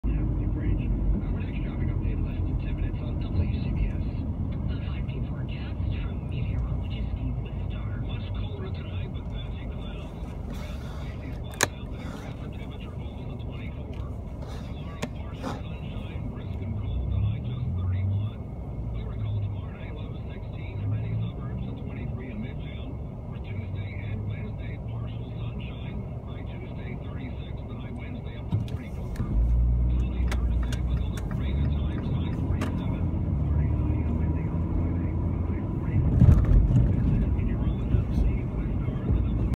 Holland Tunnel West (take 13): several vehicles including a Forester openly running the red light
several vehicles including a Forester openly running the red light please check out my twitter feed @thcsoy